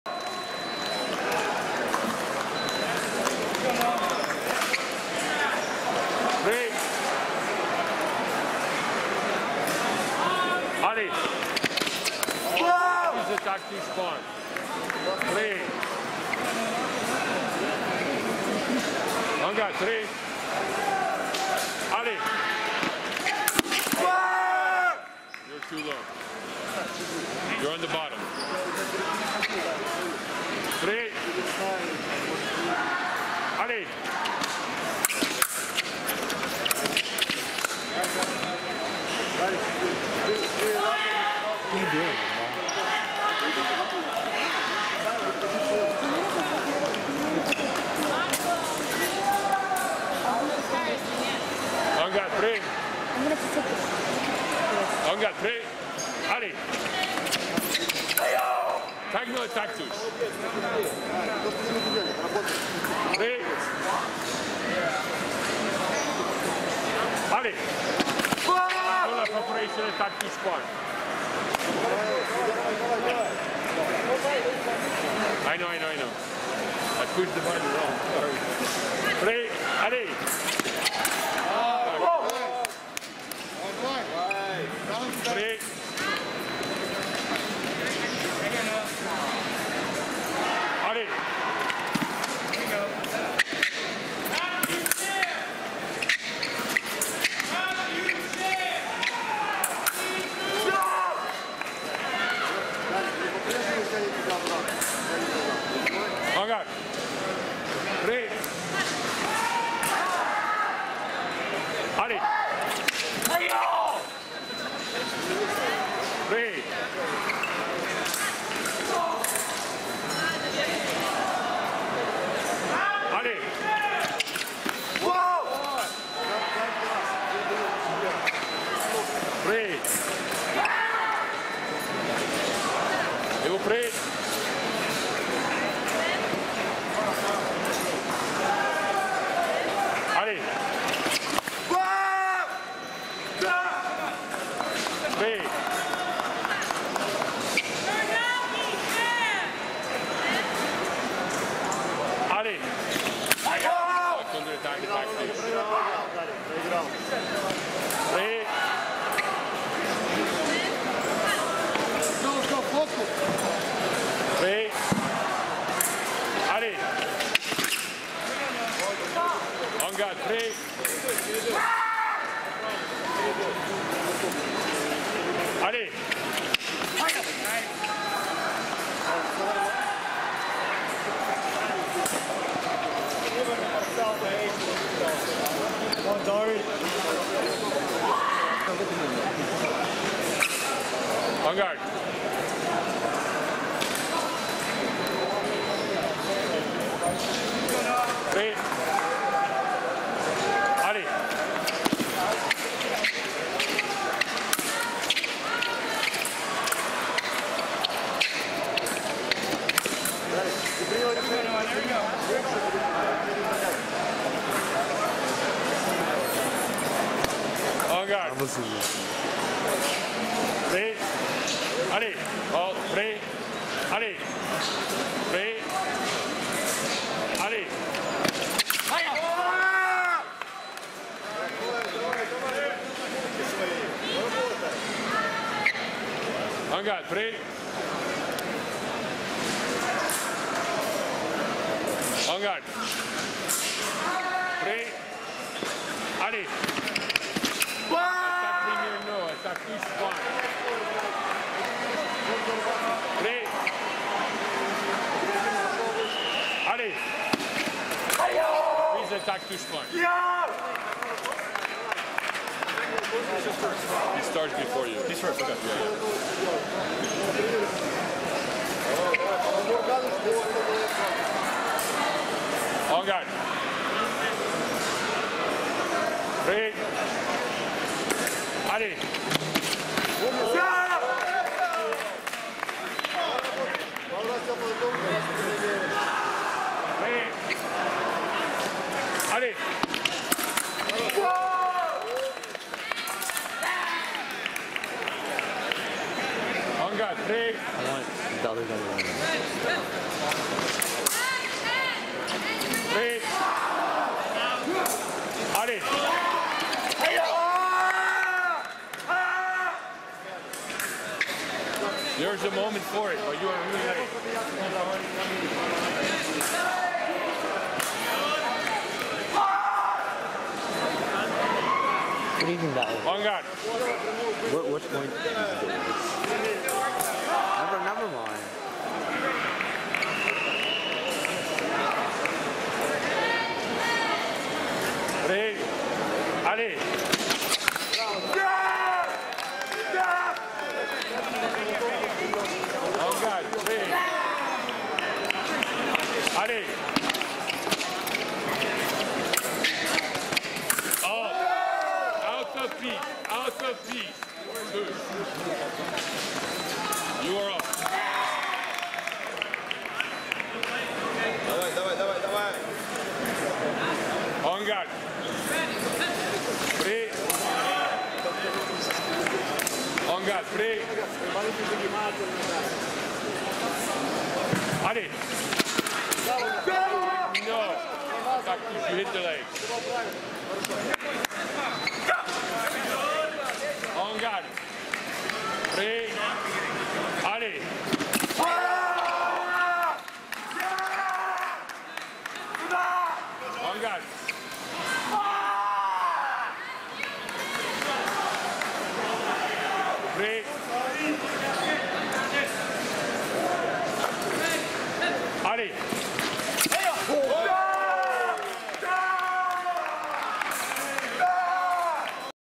Summer Nationals 2022 SMS - L128 - Noah Te Velde USA v Darii Lukashenko UKR
This is a L128 bout in the senior men's sabre event at the 2022 Summer Nationals in Minneapolis, Minnesota. Noah Te Velde of USA is on the left and Darii Lukashenko of Ukraine is on the right.

Thank you to Nicolas Wang for filming this bout!